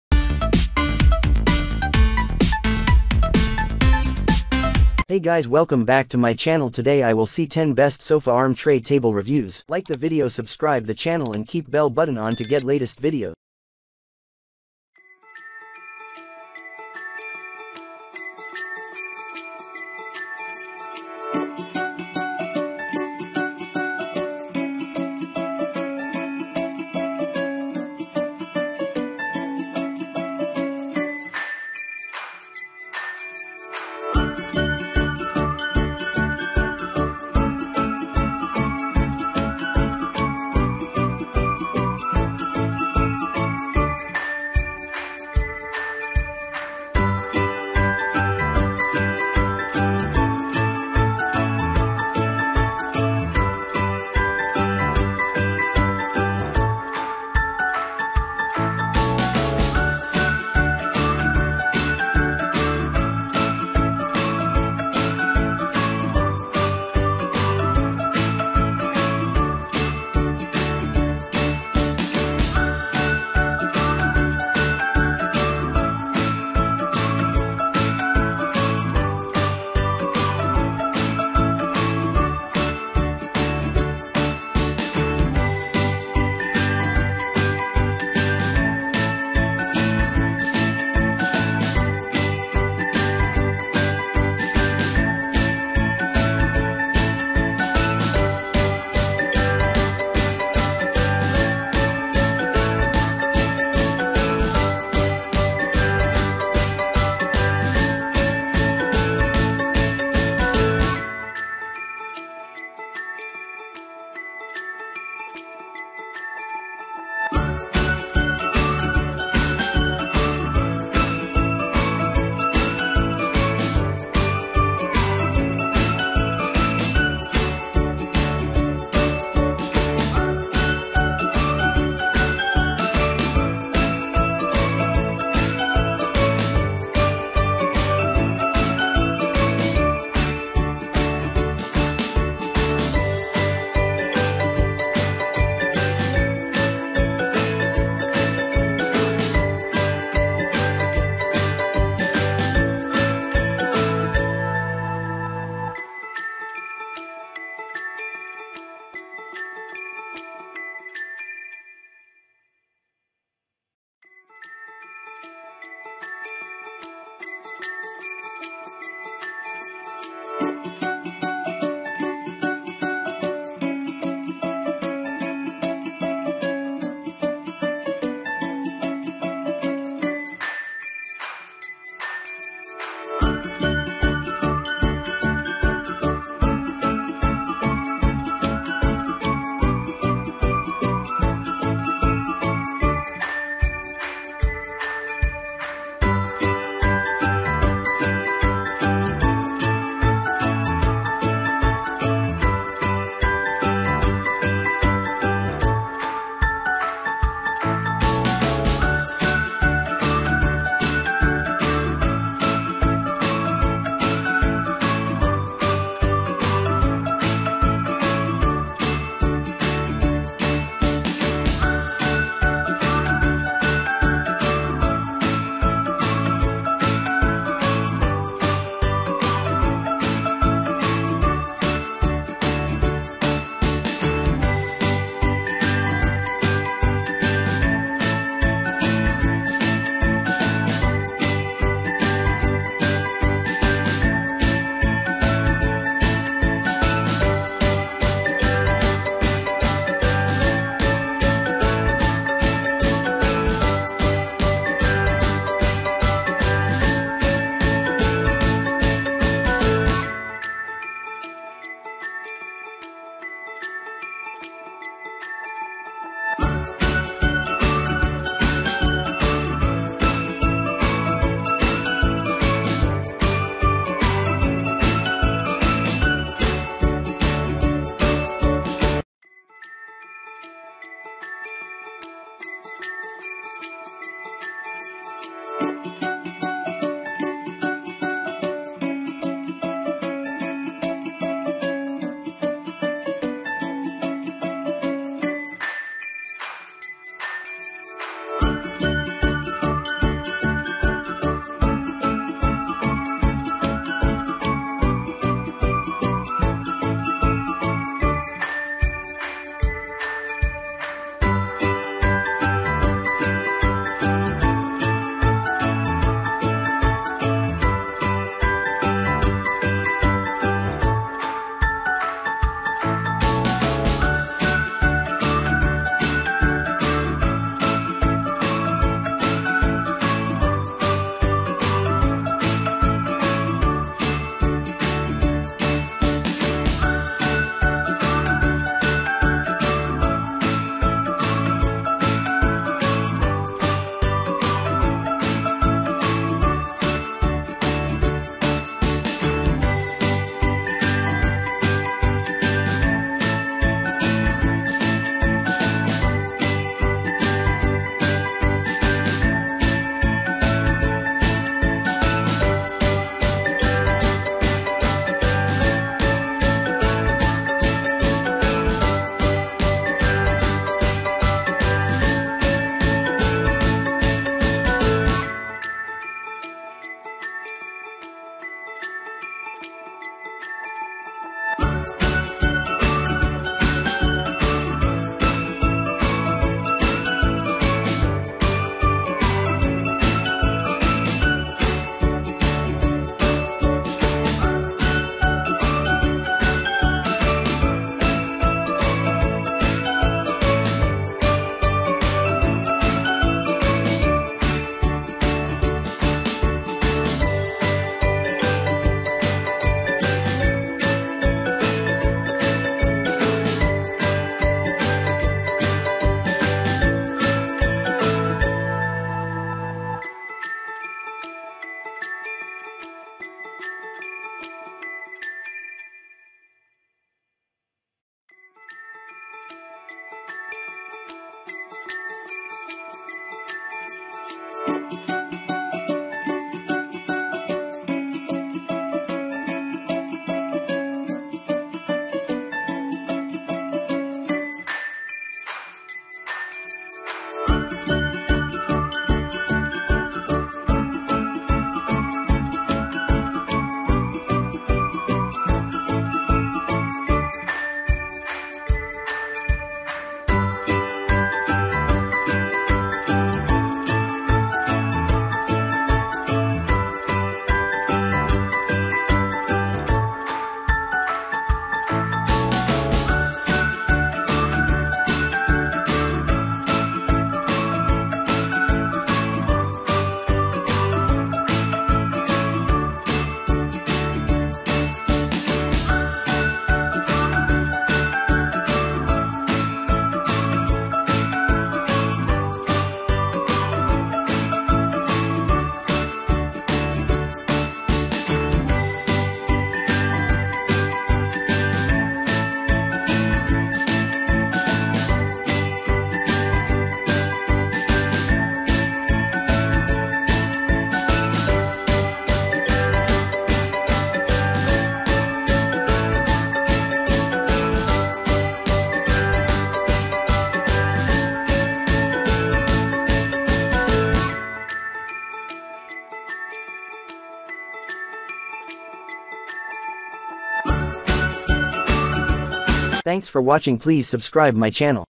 10 Best Sofa Arm Tray Table Reviews 2022 - Amazon Must haves
In this video about, ✅ 10 Best Sofa Arm Tray Table Reviews 2022 - Amazon Must haves

➡️ 𝗣𝗿𝗼𝗱𝘂𝗰𝘁 𝗪𝗶𝘁𝗵 𝗕𝘂𝘆𝗶𝗻𝗴 𝗟𝗶𝗻𝗸𝘀 :

▷ Sofa Arm Clip Table
▷ Arm Table Clip On Tray Sofa Table for Wide Couches
▷ U NEATOPA Couch Remote Caddy Control Holder Sofa
▷ LENUE Bamboo Sofa Arm Tray
▷ Arm Table Clip On Tray Sofa Table for Wide Couche
▷ EHE Sofa Arm Tray Table for Couch FlexibleFoldable
▷ Bamboo Sofa Tray Table Clip on Side Table for Wide Couches Arm
▷ Couch Arm Tray Table Smatdecis Sofa Arm Clip Table
▷ Vesuna Sofa Armrest Clip Tray Table
▷ Sofa Armrest Tray Non-Slip Silicone Patented Solid Wood

About This Video :buy sofa tables in usa,best sofa arm trays,armrest tray table,sofa arm table,armrest table ikea